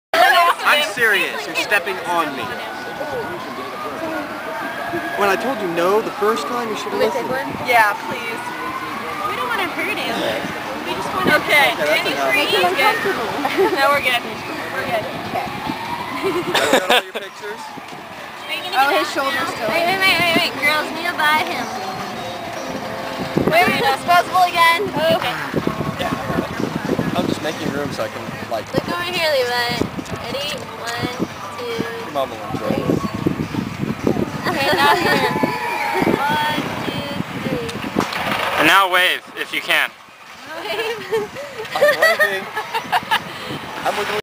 Sand Trap
Levi dug dug himself into a hole on the beach only to find himself trapped and being stepped on moments later